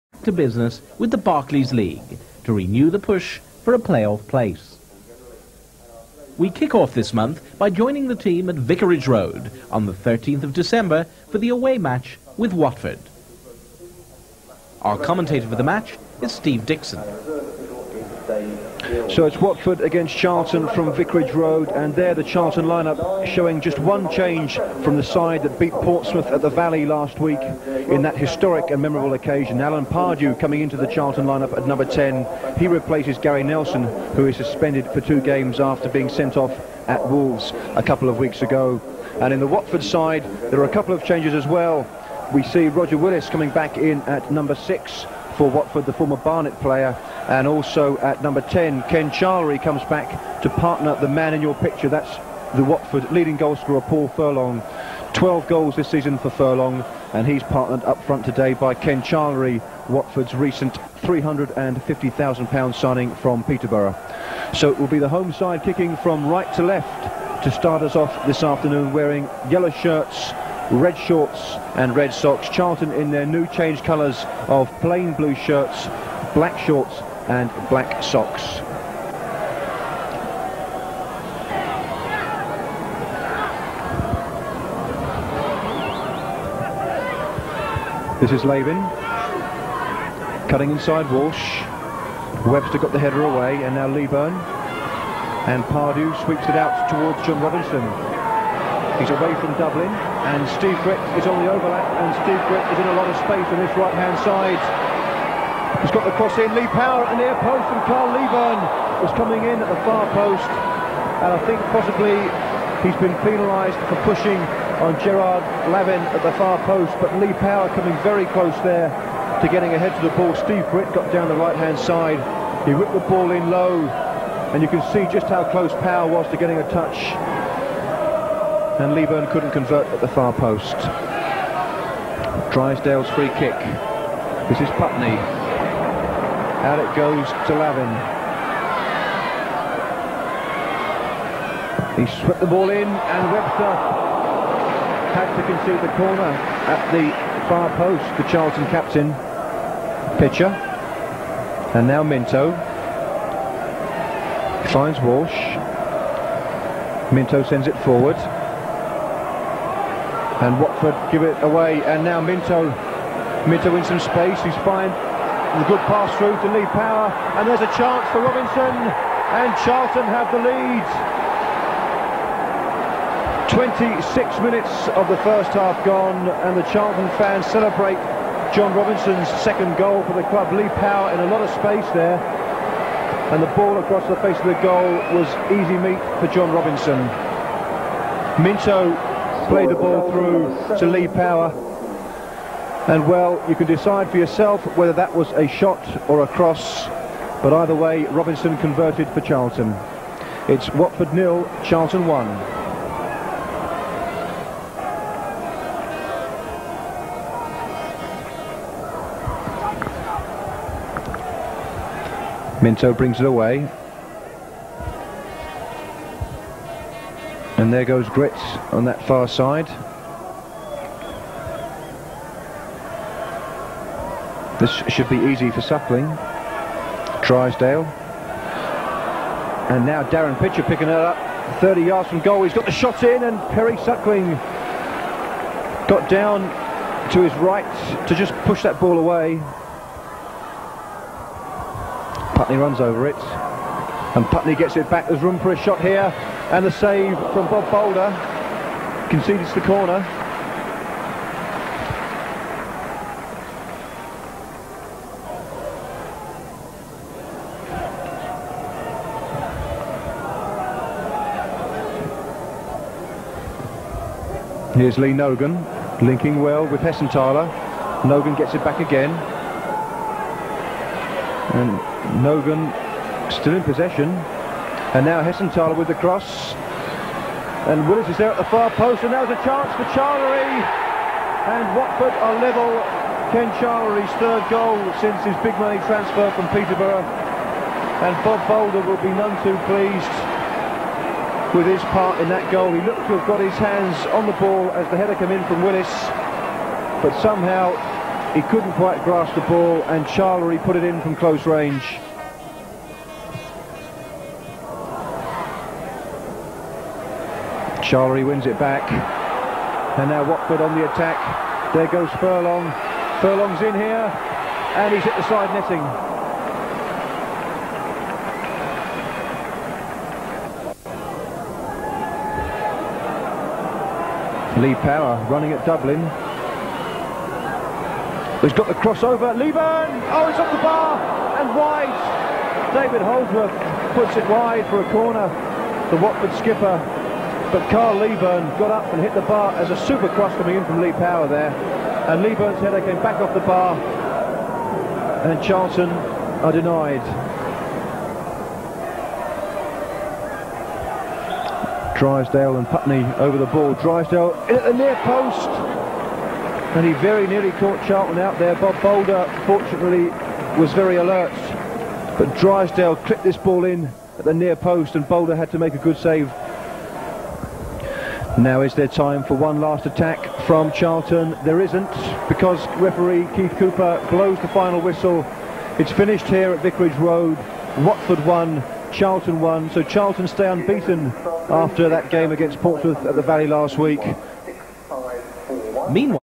192/93 Watford v Charlton Athletic (Highlights 6min)
13th December 1992 (Sun. – 3 p.m.) at Vicarage Road (6,541)
Watford 1 (0) (Charlery 52): Suckling, Lavin, Holdsworth, Dublin, Drysdale, Hessenthaler, Putney, Willis, Nogan (Bazeley 62), Charlery, Furlong. Sub. (not used) Alsford.
Charlton Athletic 1 (1) (Robinson 26): Bolder, Pitcher, Minto, Gritt, Webster, Balmer, Robinson, Power, Leaburn, Pardew, Walsh (Grant 35). Sub. (not used) Bumstead.
Referee: Keith Cooper (Pontypridd (Mid-Glamorgan)
Order of goals: CA W
Booked: Gritt (56), time-wasting; Webster (71), for a foul; Drysdale (33), kicking ball away.
Television: Match covered ‘live’ by LWT.